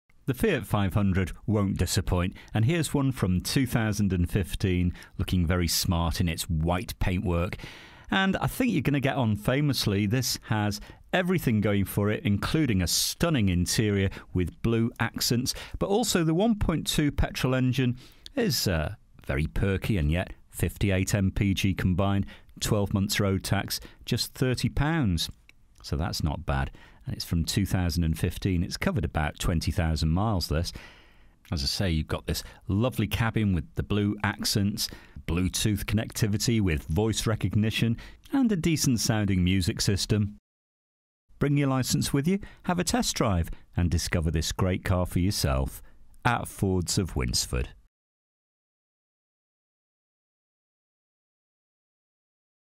FIAT S (2015) 1.2 S 3dr
DETAILS:
First registered: 01/03/2015
Current mileage: 20070
Registration: WV15LXP
Colour: White

This used car is for sale at Fords of Winsford Car Supermarket in Cheshire, UK